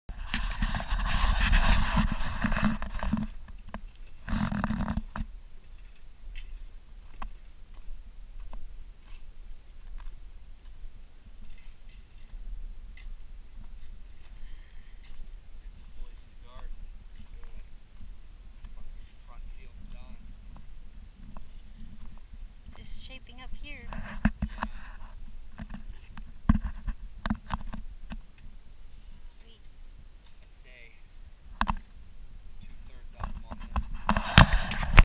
Earthbag eco house foundation: Laying the gravel for the drainage ditch
Grabbed our shovels and got to work digging the drainage ditch for the Earthbag house.